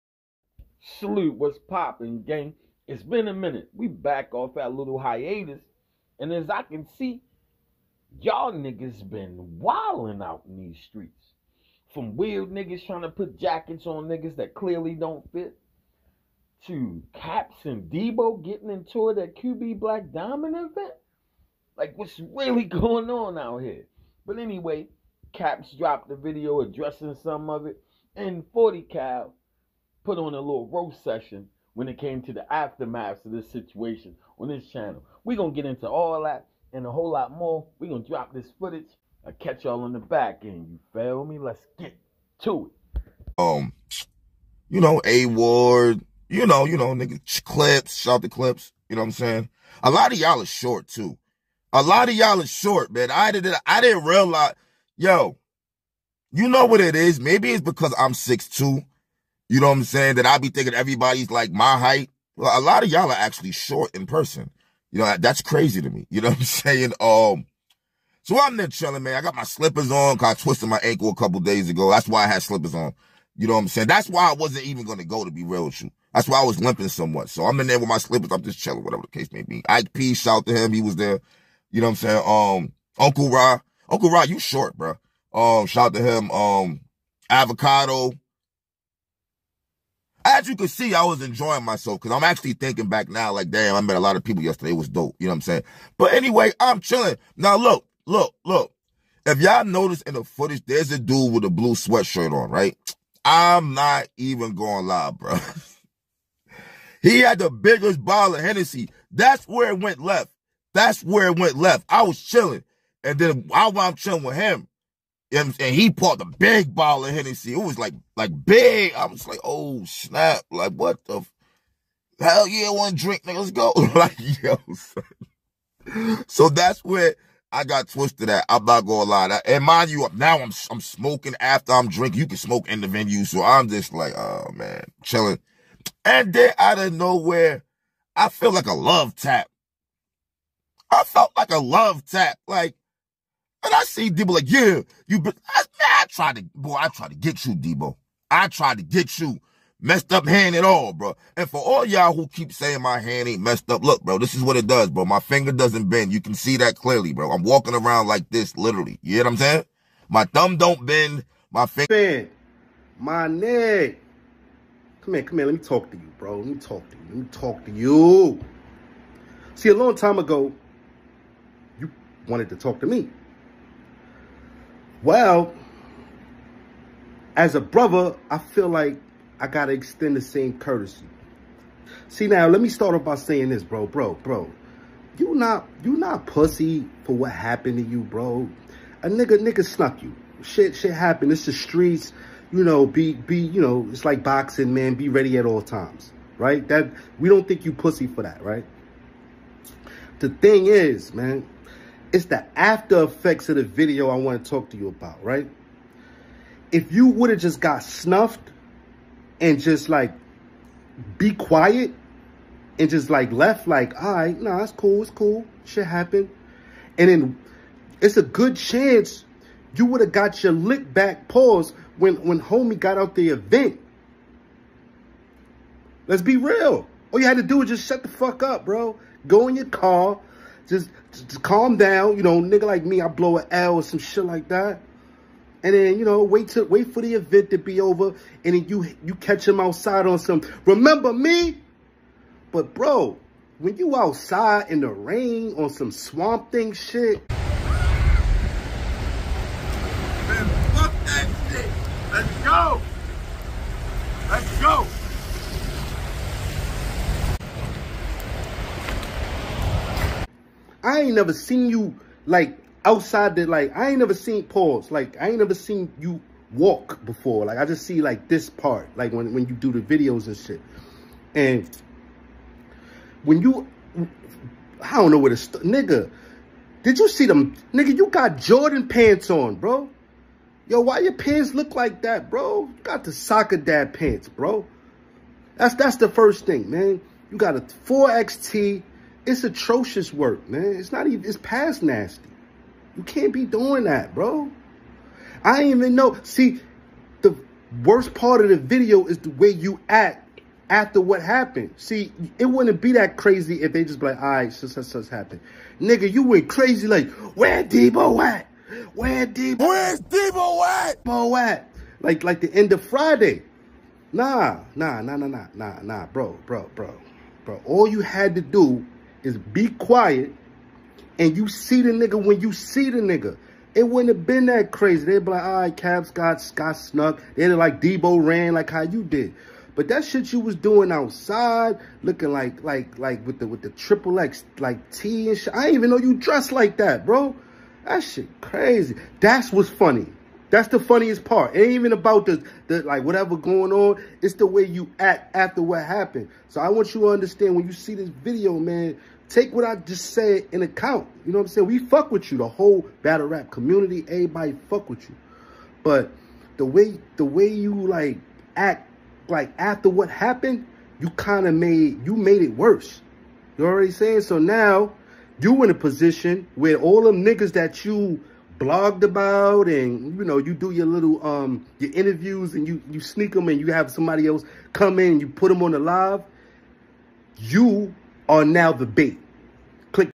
Shocking Update: Angryfan vs. Debo + 40 Cal message to caps 😱
Commentary and video on the alleged Altercation that happened at QB black Dimond event between angryfan007 and debo plus 40cal Roast welcome to Gen-X Media hip-hop and Battle Rap